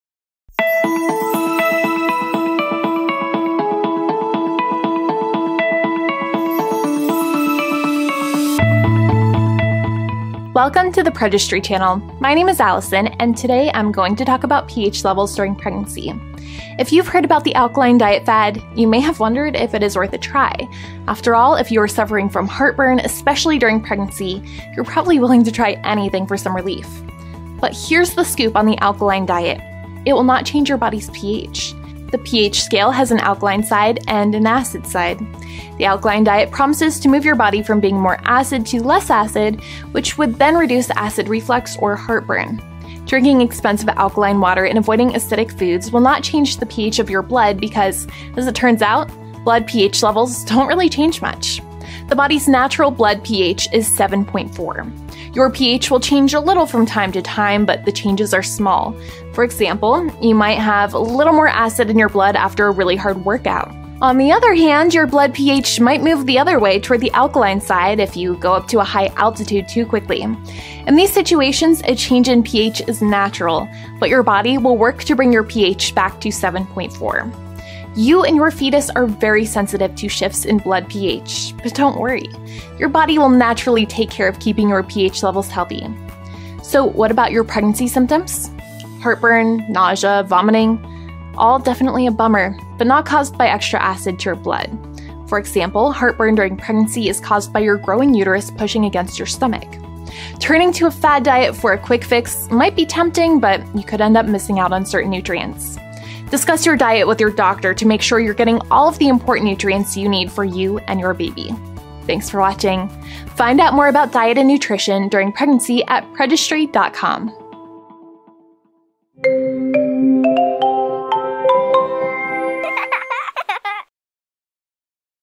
pH in Pregnancy - Why Your Diet Will not Change it?
Hello, today I’m going to talk about pH levels during pregnancy.

You may have heard about a fad called the “alkaline diet”. It’s sold to pregnant women as the solution to problems like acid reflux, also known as heartburn. The idea is that, because alkaline is the opposite of acid on the pH scale, an alkaline diet will relieve heartburn and other health concerns. That sounds nice, but you cannot significantly change the pH of your blood. In fact, you don’t want to!

The cool thing about your body is that it will keep its pH levels steady no matter what you eat. The pH level in your blood will naturally be 7.4. You may be slightly higher or lower sometimes, but your body will always work to get back to normal. Any heartburn that you may experience during pregnancy isn’t due to extra acid in your blood. It is caused by your growing uterus pressing on your stomach. Instead of turning to a fad diet, talk to your doctor to make sure you’re getting all of the important nutrients you need for you and your baby.